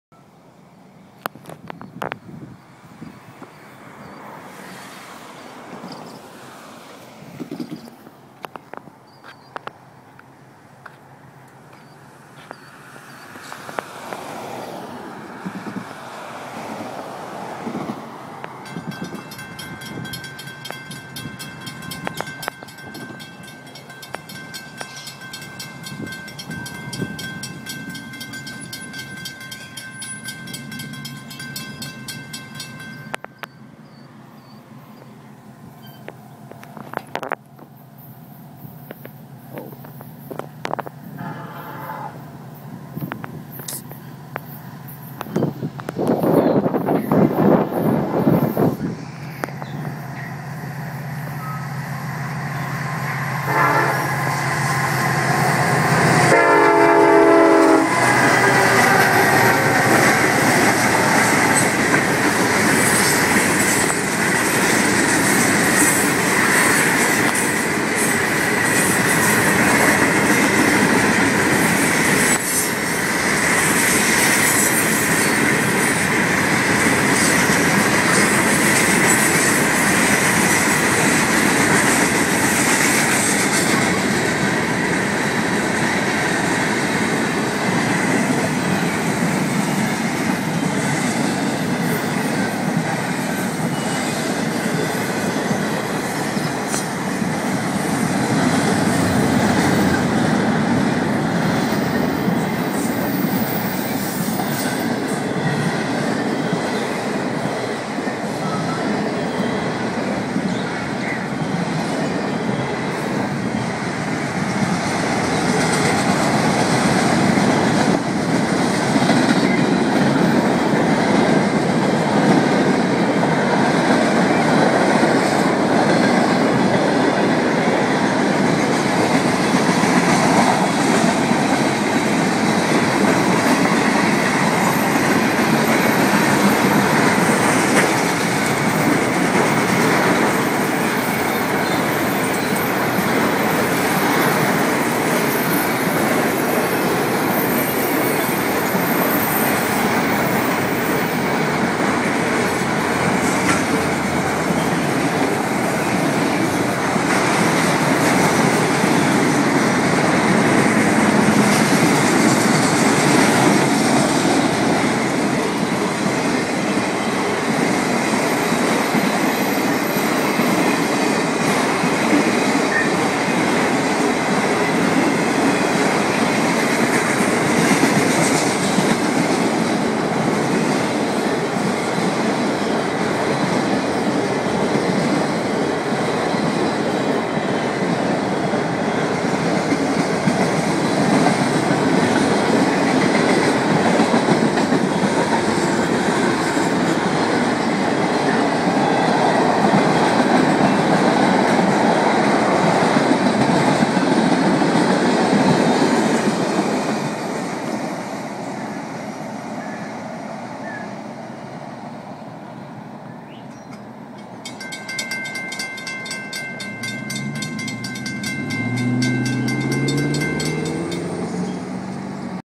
FEC TRAIN 202#169 WITH ECH AND JIM
Northbound fec engines 104 and 716  5/312 Eot past mp 198 at 5;45p